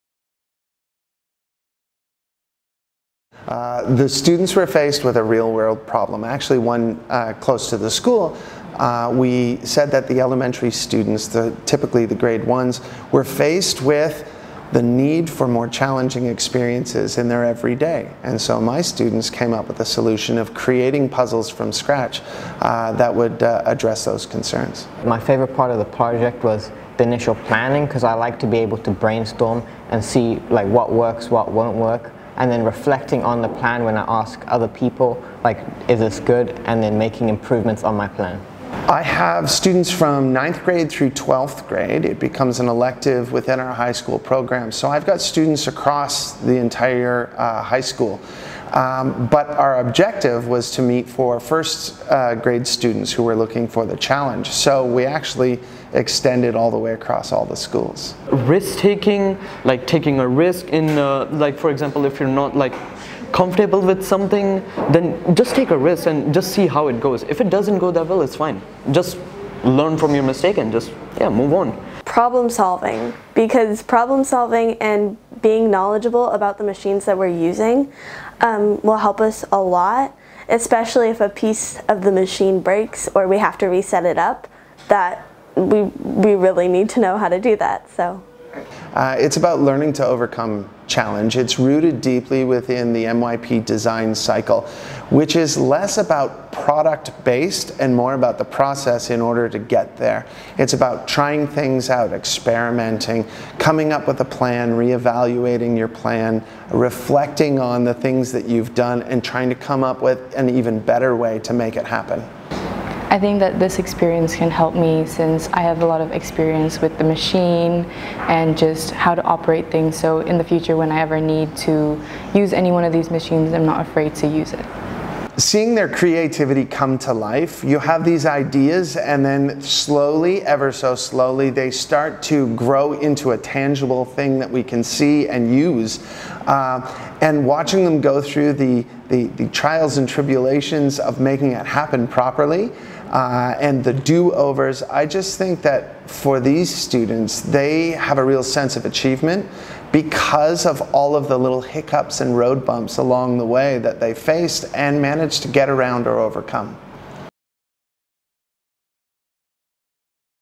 High School Product Design Technology Class | Mont'Kiara International School (M'KIS)
Our high school students from the Product Design Technology class are given a problem or issue to resolve by designing a product-based solution for what they face. Students fabricate their solutions using the resources and machinery available to them, in this case, plywood and their imaginations. They are all at an introductory level with their knowledge and skills in this area, yet, some amazing final products have been created.

The wood puzzles were tested by our first graders, and they loved it! Thanks to Mr Jonathan for conducting such a fun, interactive and innovative project.